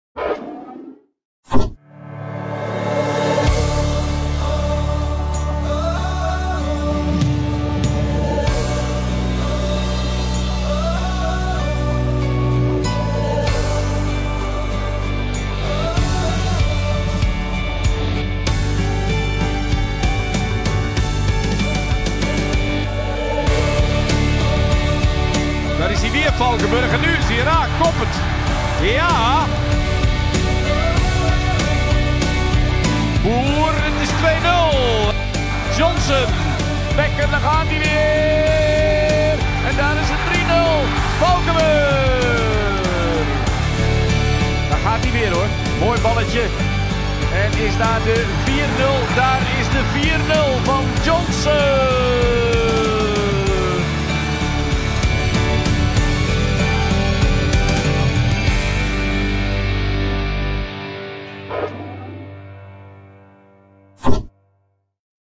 'Aftermovie' ADO Den Haag - PEC Zwolle
Heerlijke beelden van de laatste wedstrijd van 2017.